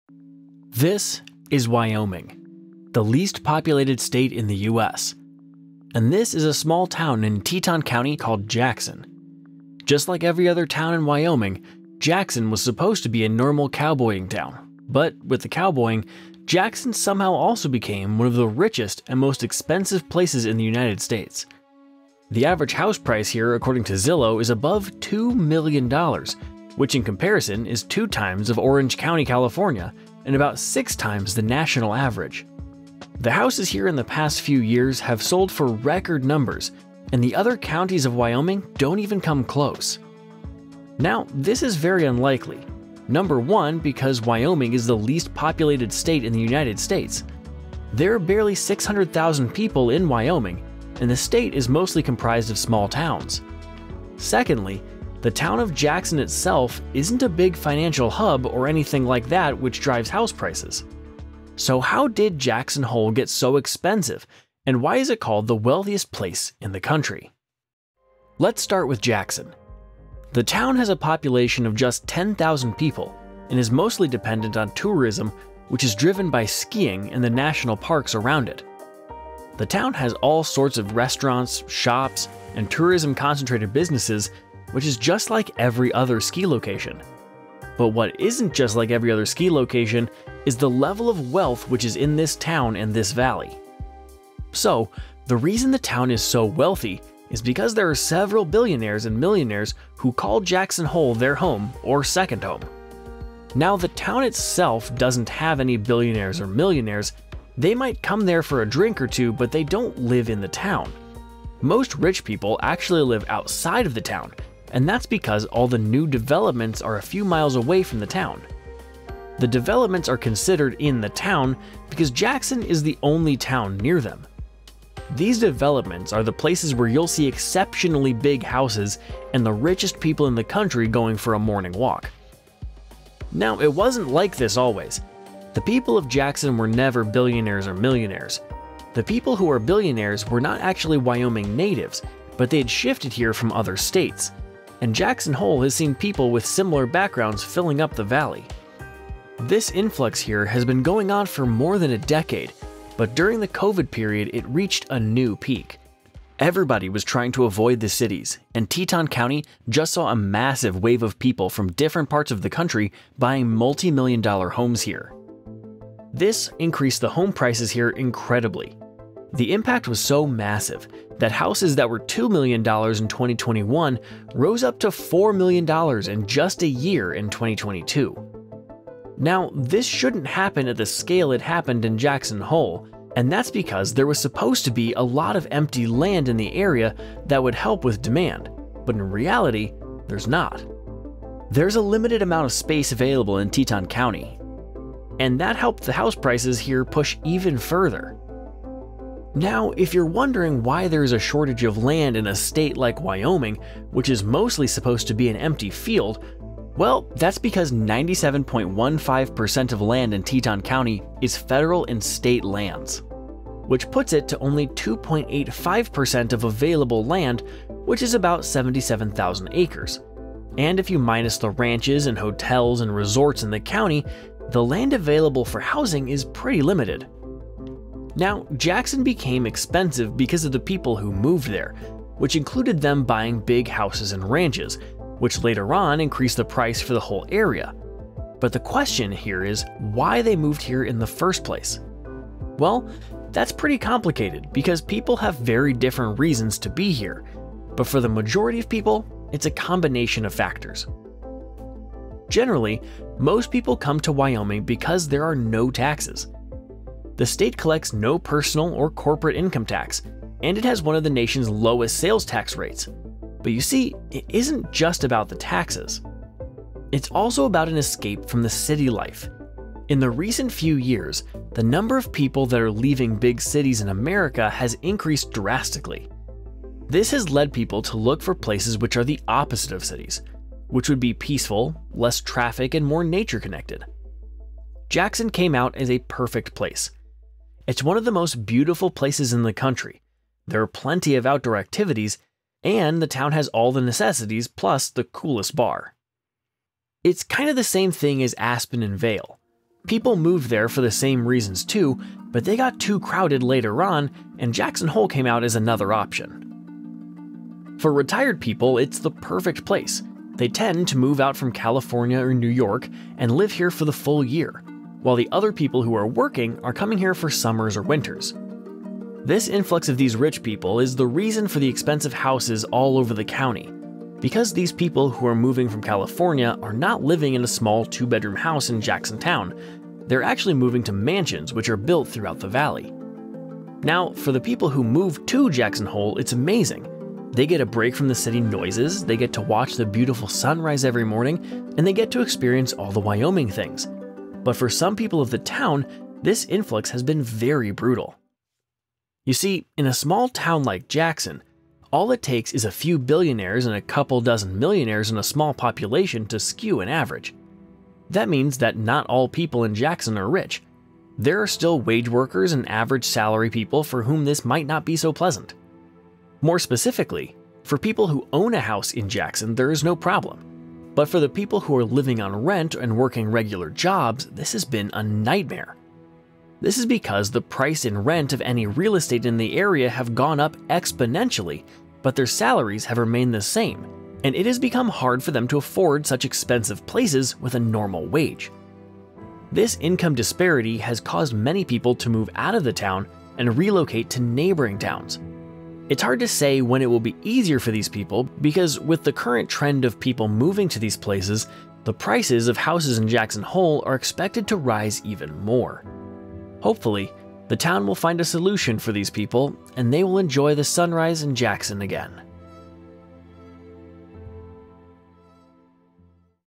Why This Small Town In Wyoming Is So Rich
Jackson, wyoming is one of the richest and most expensive place in the united states, however this is a very small town with very little population driven by tourism.